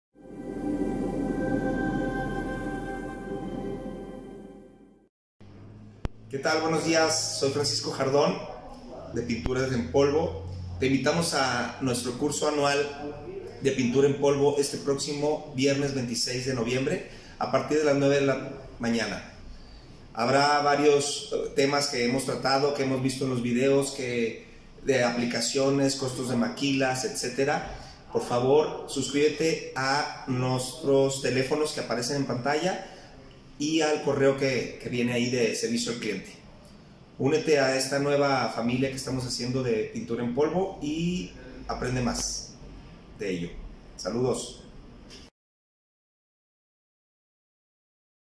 ¡EL CURSO DEL AÑO!: APLICACIÓN DE PINTURA EN POLVO 2021
Te invitamos este 26 de noviembre a nuestro seminario en línea, en el que se tratarán diferentes temas relacionados a la aplicación de pintura en polvo. Registrate por medio del correo o por teléfono. TE ESPERAMOS!!